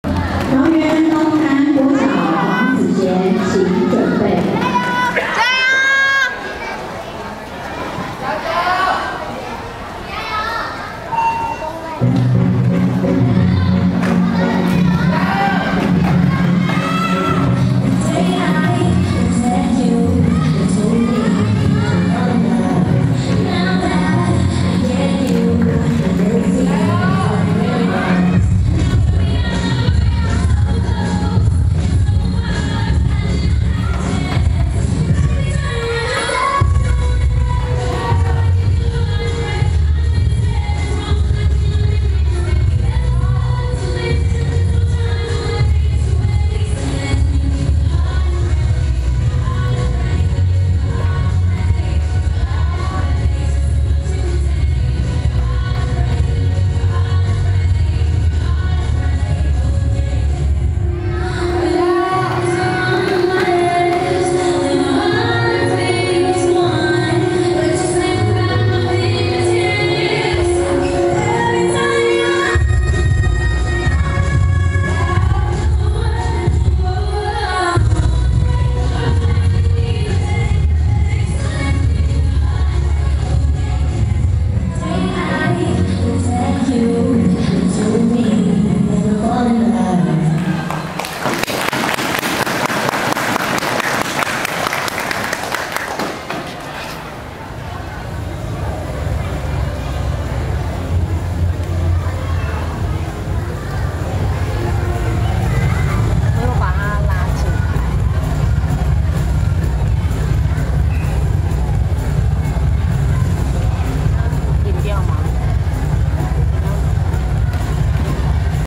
106年全國有氧體操錦標賽_陳禹彤
高年級女子單人組第四名